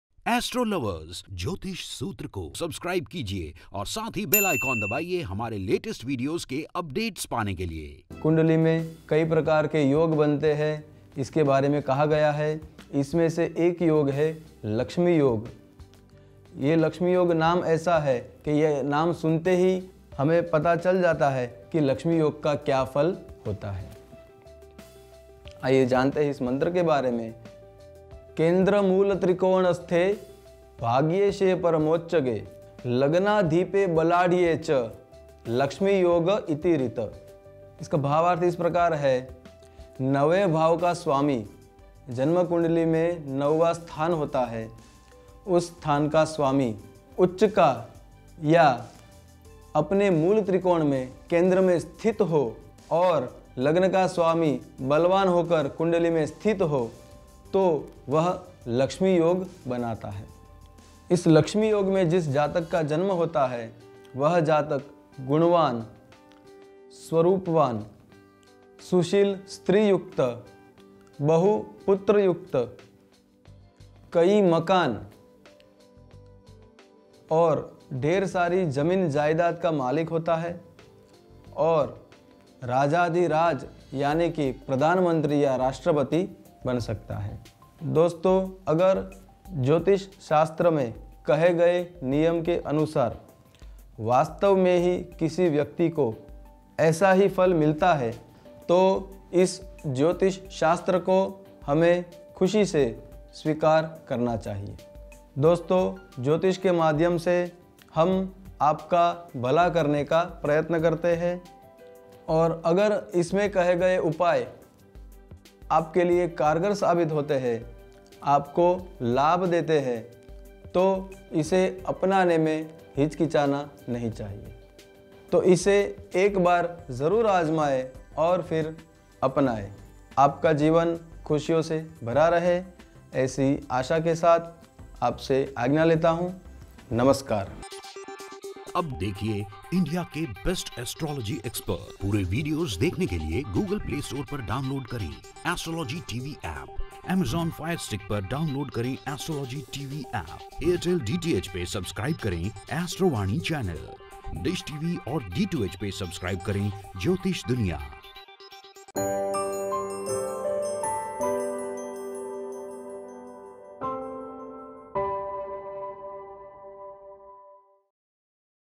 कुंडली में यह योग आपको आमिर बना देगा | Yoga In Your Kundali That Indicates Wealth
Astrologer Bhavesh Dave tells about Lakshmi yog in our Kundali that brings wealth & abundance in life.